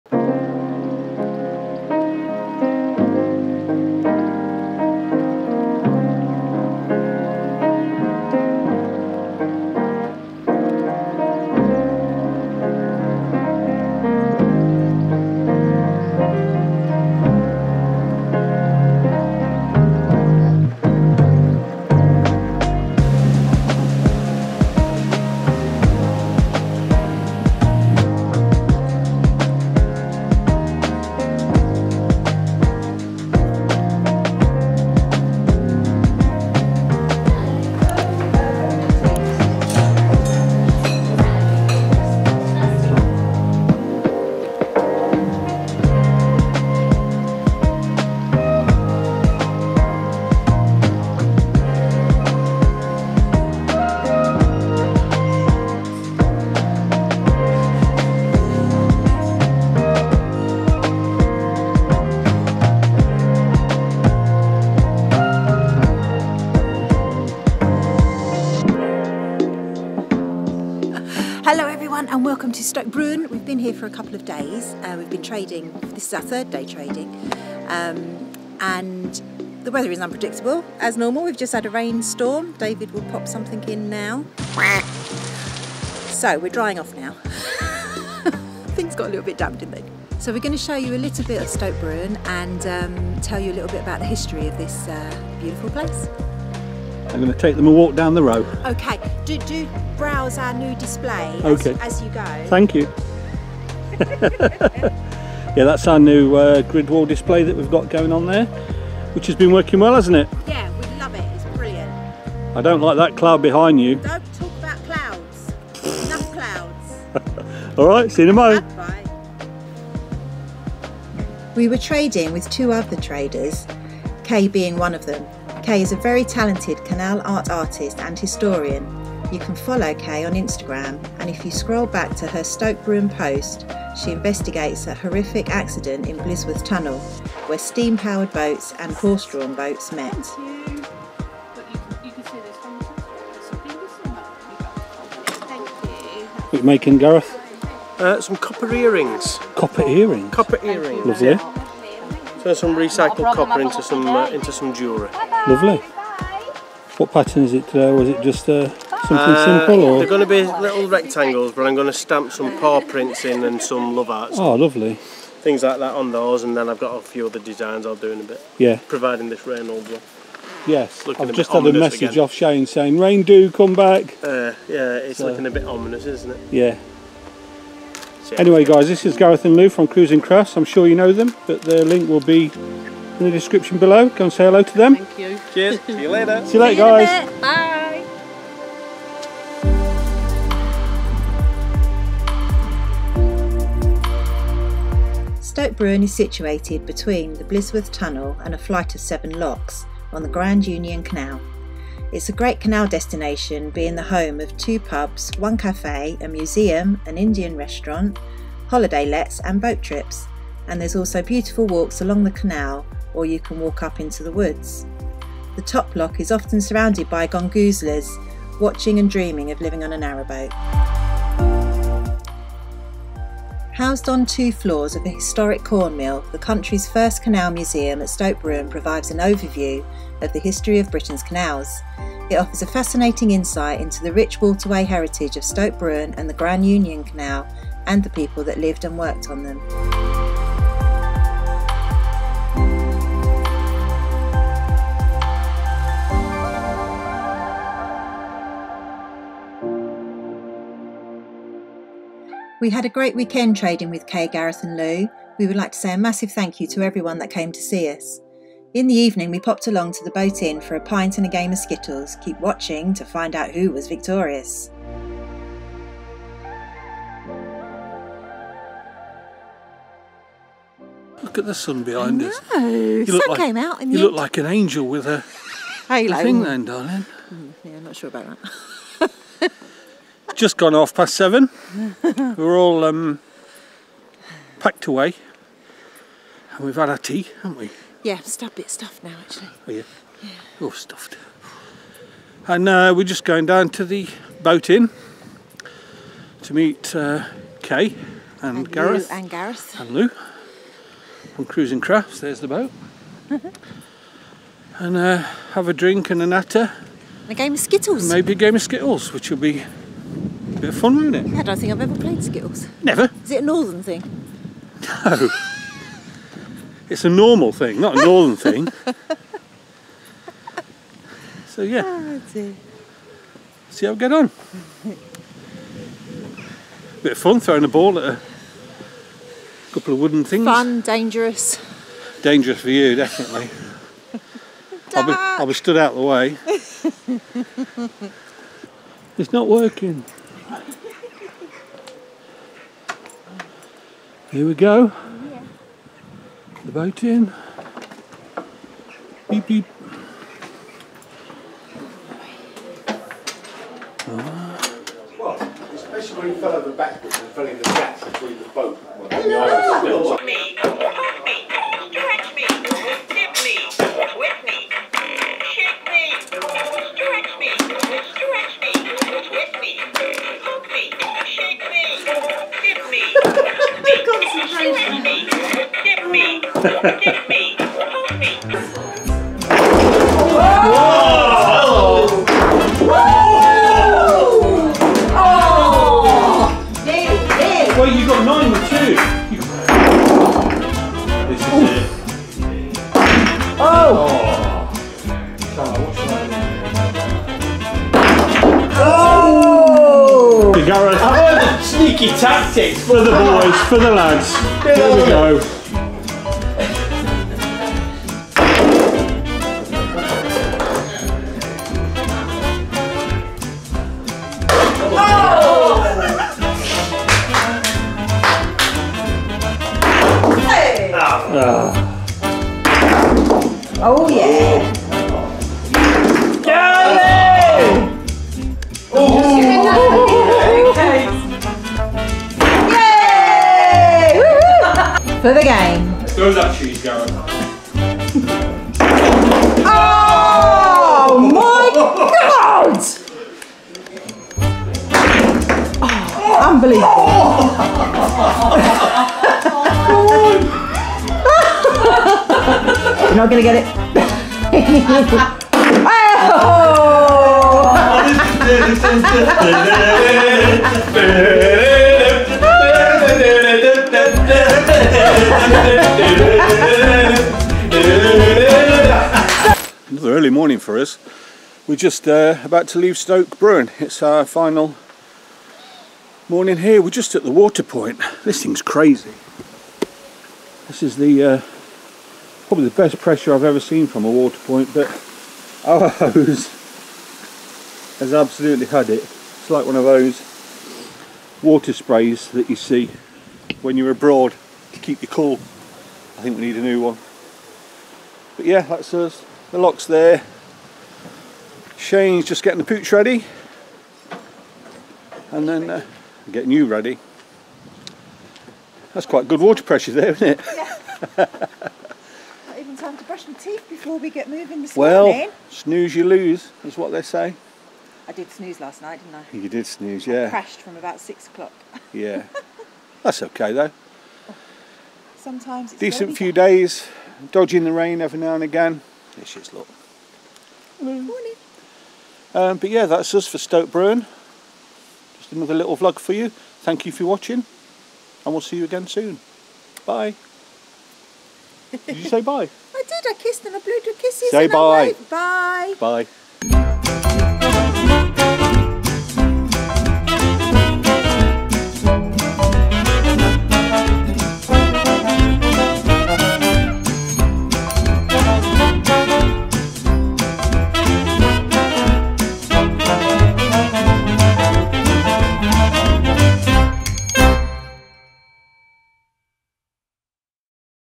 Explore the Hidden Gem of Stoke Bruerne and Narrowboat Trading
We have a pop-up at Stoke Bruerne with Kays Canal Arts and Cruising Crafts as well as our Stoke Bruerne Skittle Tournament 2023, Please don't forget to drop us a like and give us a subscribe if you're not already.

Kays historical tale from Blisworth tunnel